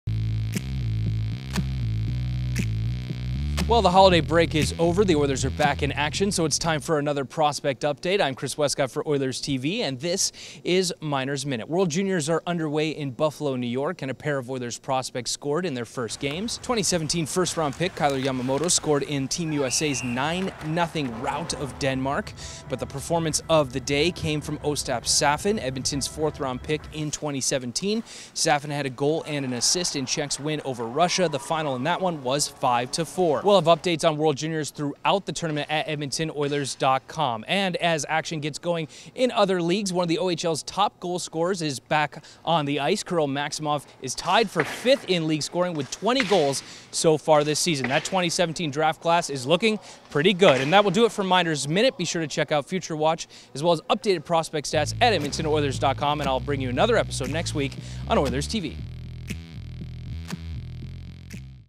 MINORS MINUTE | Yamamoto/Safin at WJC
Updates on Ostap Safin and Kailer Yamamoto at World Juniors and more in this week's prospect update. Chris Wescott reports.